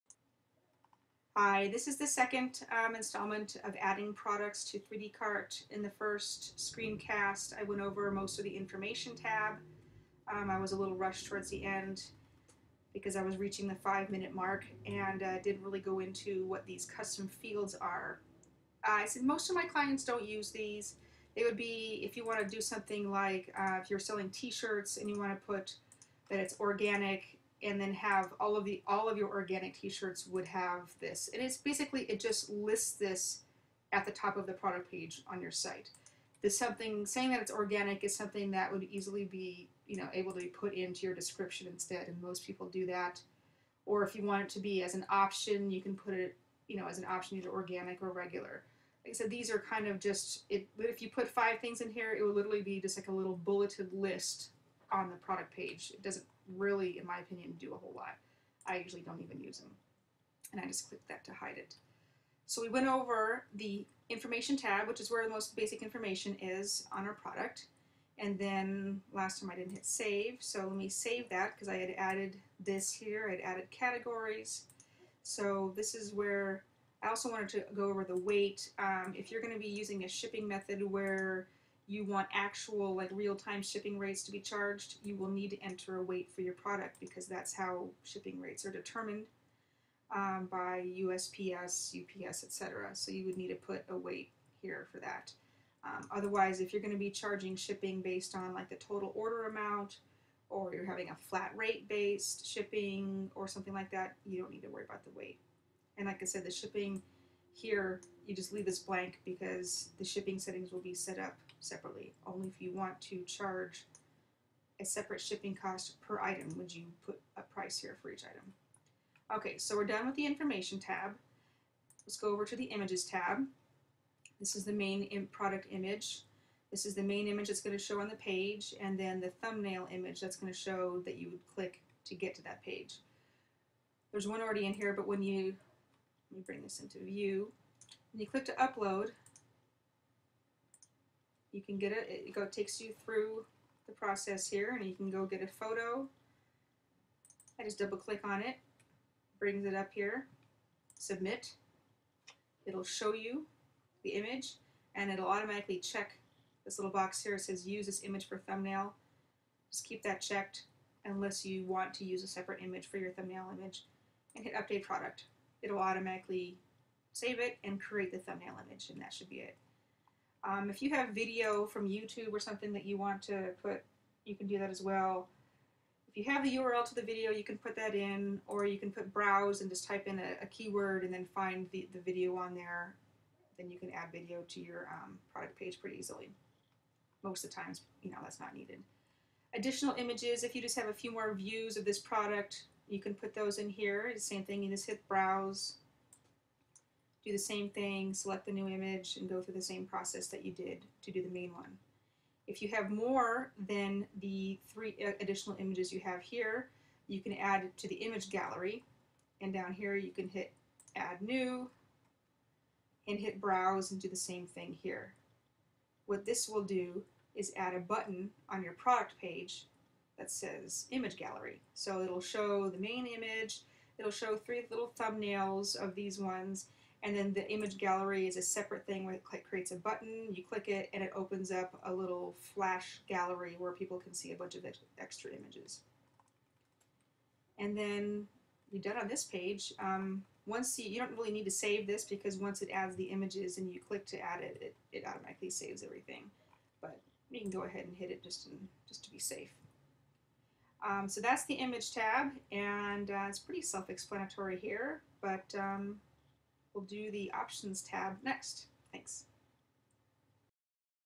How-To: Adding Products to 3dcart | Part II, the Images Tab
PRODUCTS PART 2: Basics of entering a product into 3DCart. The Images tab.